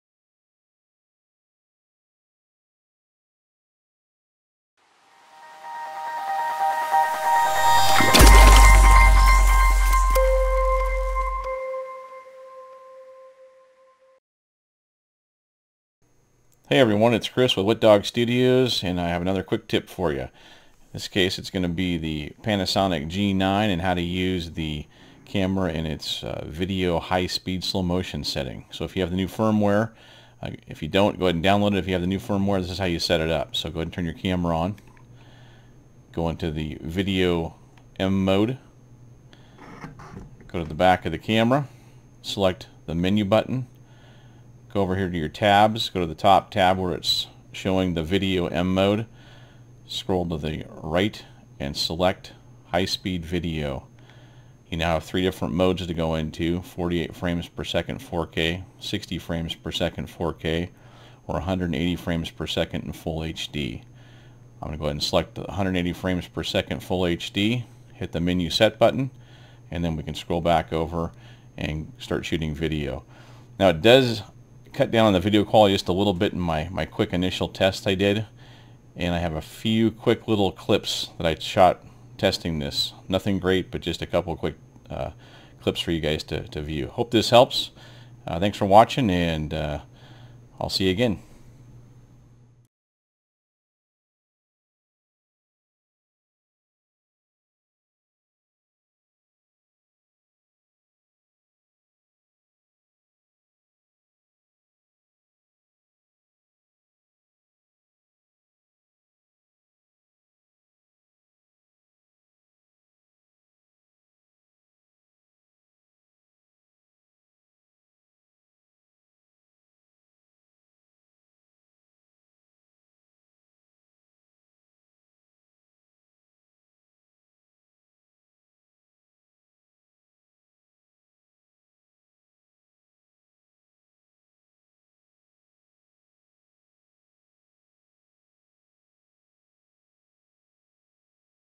Setup Panasonic G9 for "High Speed Video" Slow Motion Recording
This is a quick tip tutorial on how to setup the Panasonic G9 for High Speed Slow Motion video capture.